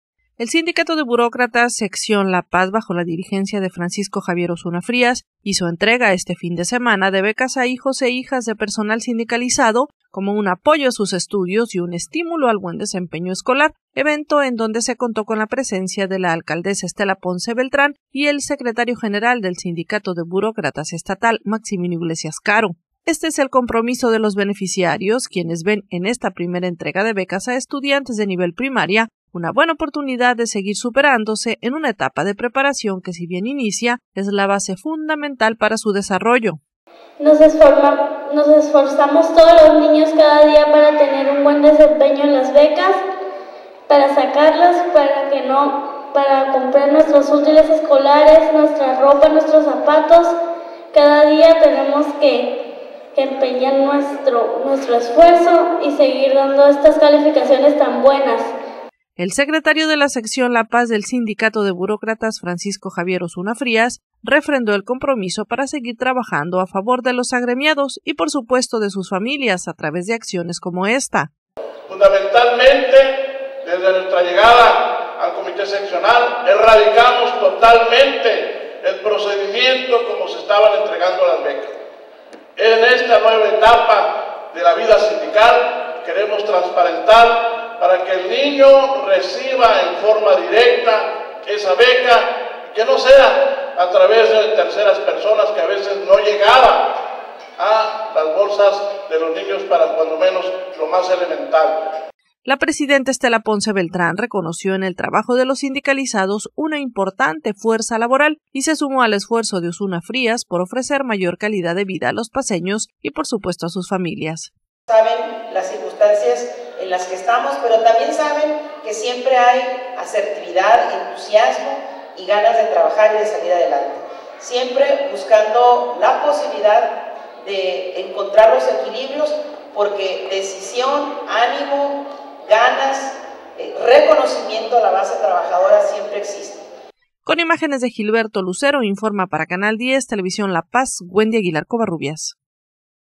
BECAS BUROCRATAS
SINDICATO BUROCRATAS ENTREGA BECAS A HIJOS DE TRABAJADORES